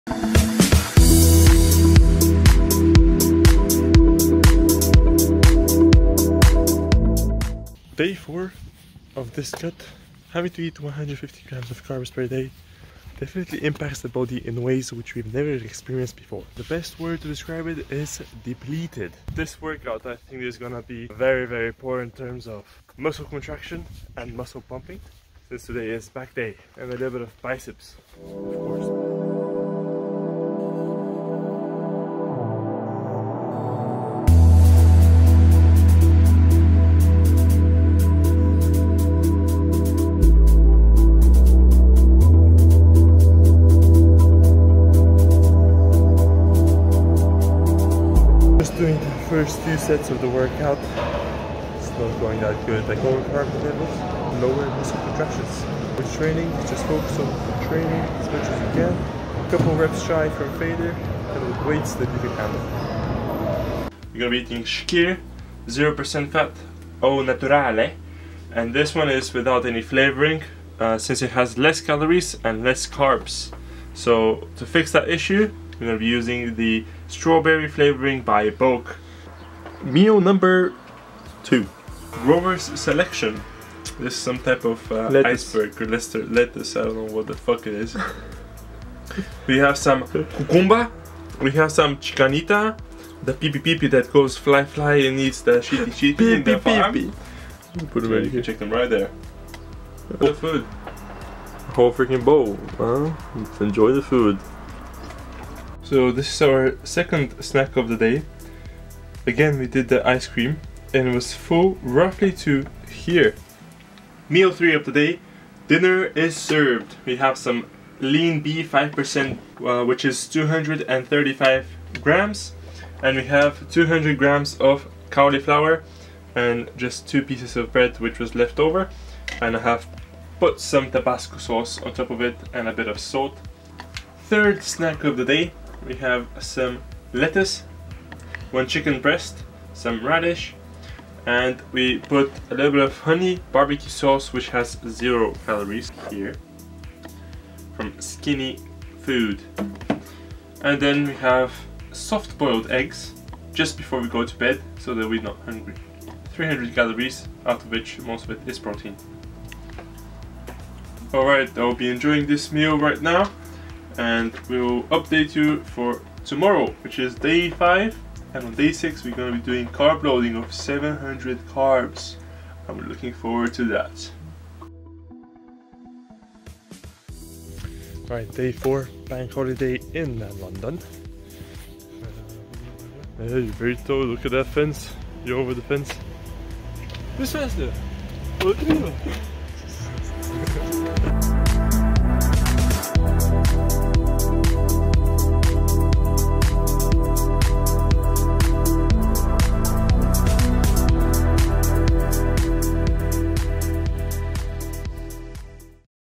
Peak Week Day 4 | Meal Plan | Lower Carb Low Contraction
Drastically dropped carbs and we felt depleted!
We couldn't feel the muscles contracting as we usually do when we're on normal carb days (200-260g).

Protein: 266
Carbs: 84
Fats: 77

NOTE: Video production quality is doodoo, but we shall improve as we stay consistent.